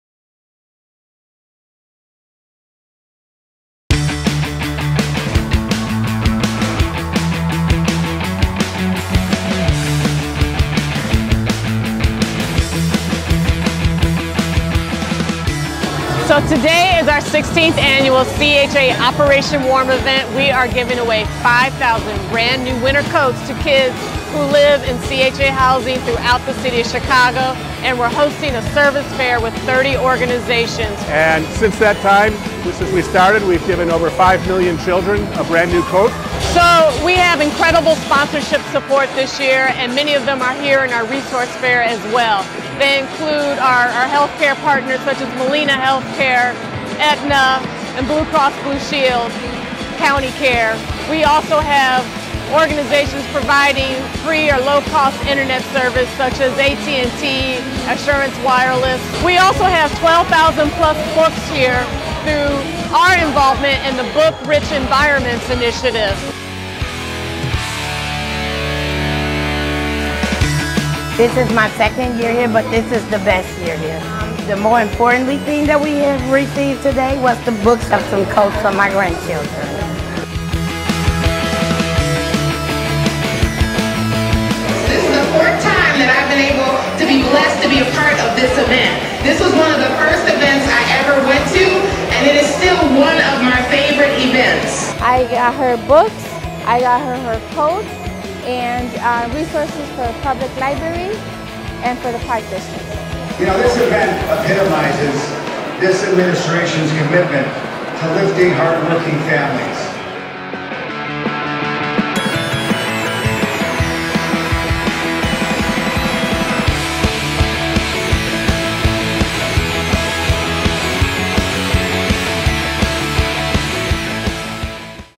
2023 CHA Operation Warm
The Chicago Housing Authority partnered with the national non- profit Operation Warm for the 16th year, handing out 5,000 new winter coats to more than 2,000 families with children living in public housing and participants of the Housing Choice Voucher (HCV) program.